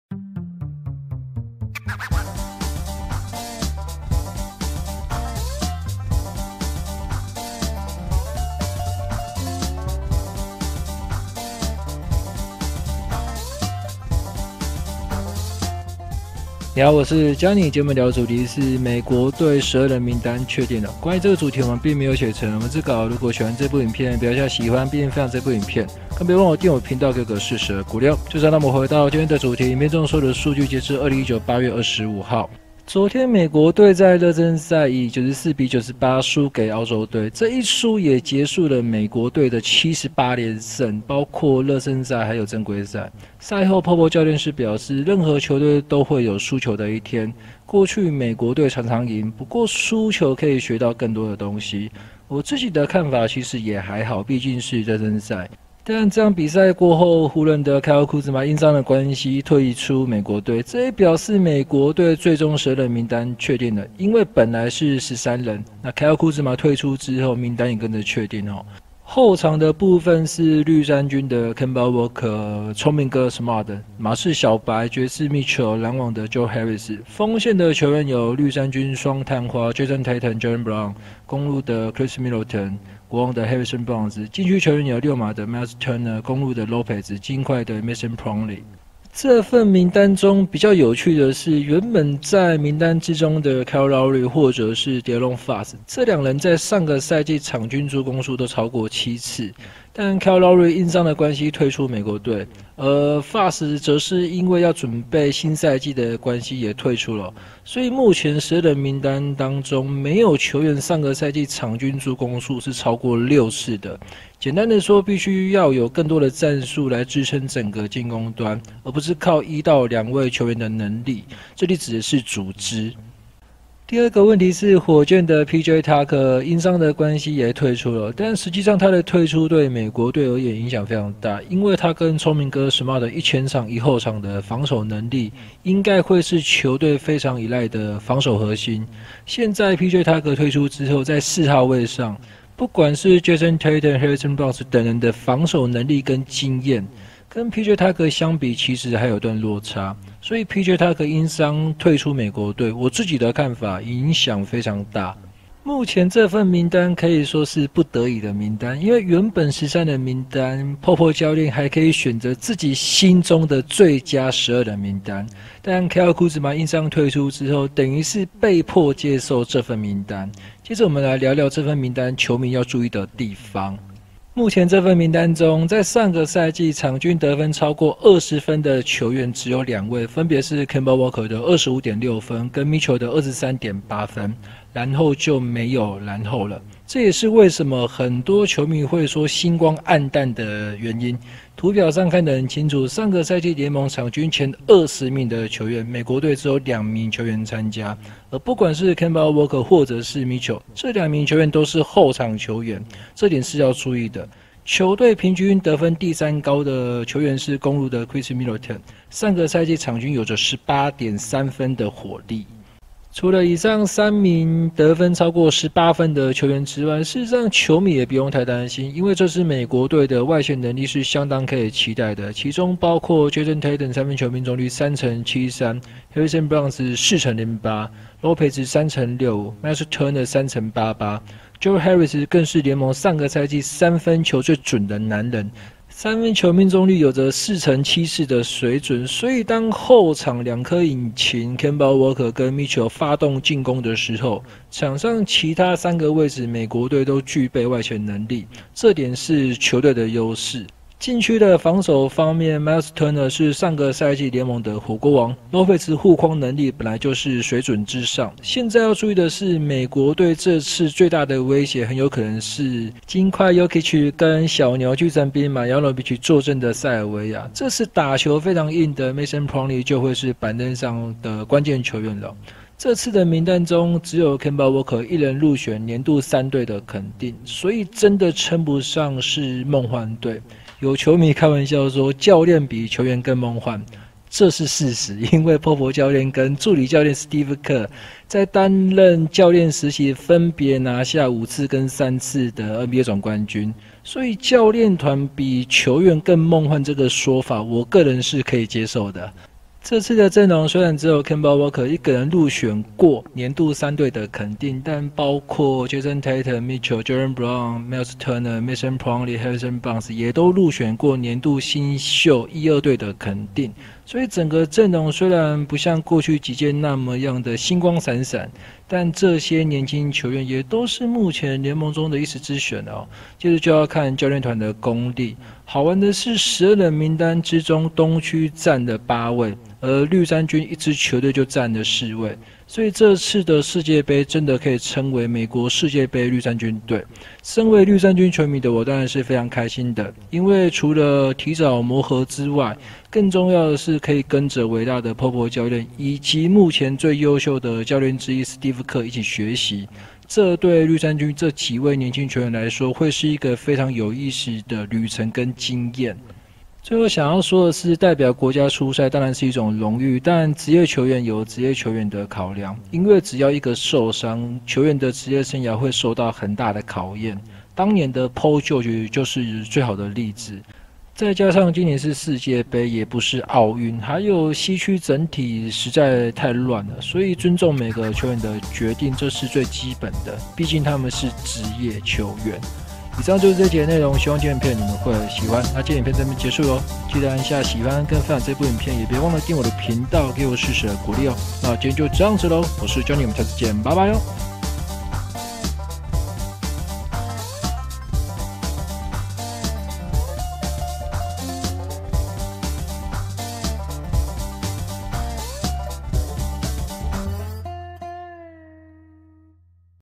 「NBA」世界盃，教練團比球員更夢幻？美國隊12人名單確定！(Johnny聊nba)
目前看起來教練團看起來是比較夢幻的，來聊聊吧！